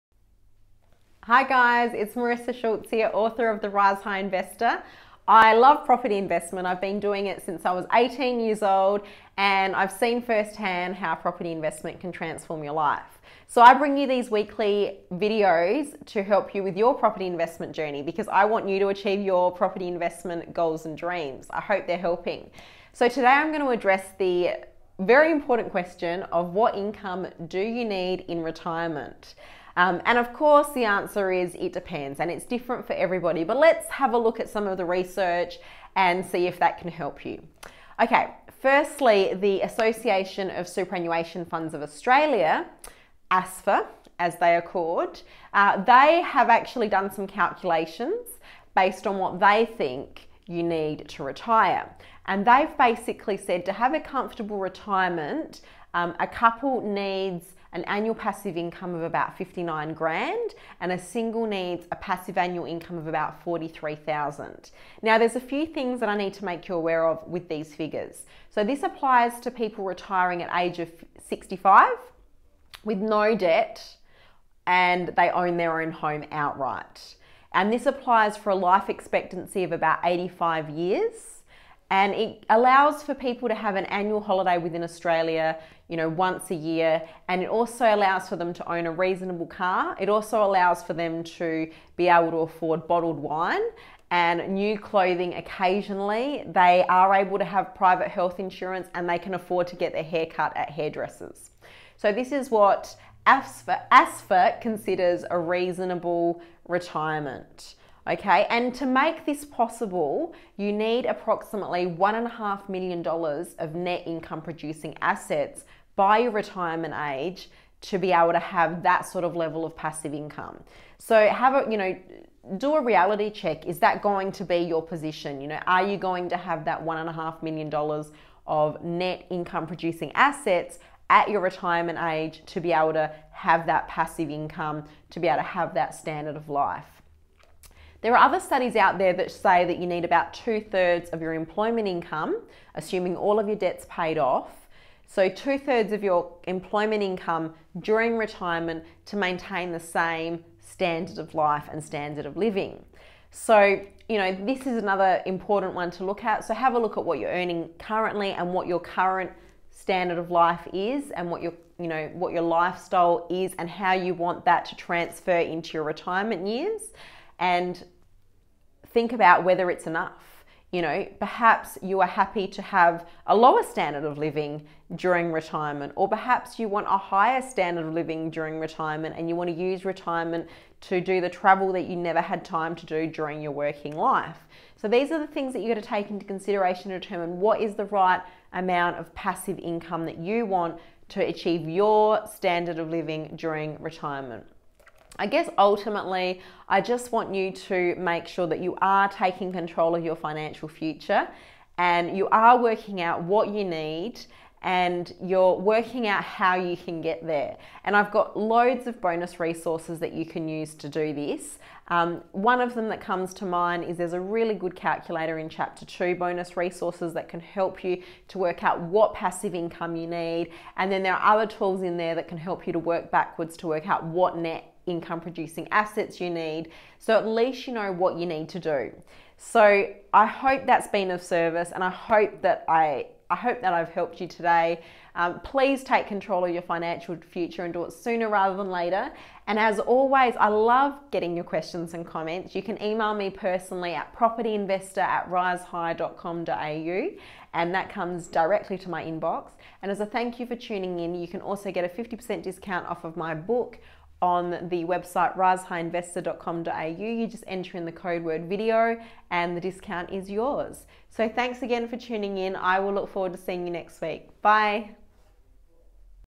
What income do you need during retirement?
Do you know how much income you need in retirement?

Marissa addresses this question and goes through research and tips on how you can live the lifestyle you desire during retirement.

Do you need a money coach to feel completely empowered to make the right money decisions for you and your family?

Video Content:
0:00 - Introduction
0:38 - What do you need to retire?
2:17 - How much employment income do you need?